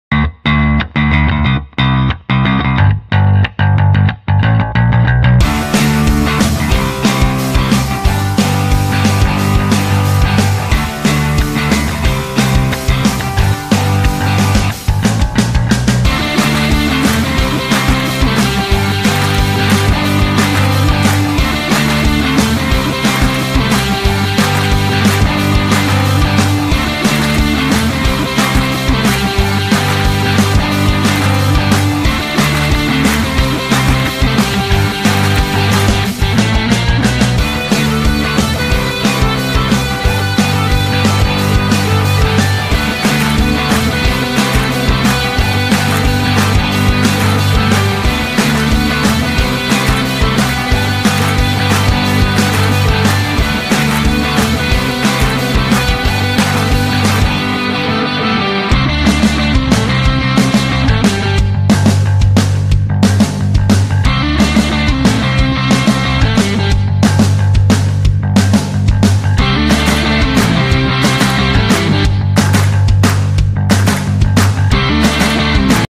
How to load/stack carton/cardboard/sheet after FFG? How Load Former work with printer/FFG?
Taiyang High Speed Load Former is designed for the latest high speed FFG(400 pic/min), allowing for the fastest load discharge times in carton industry.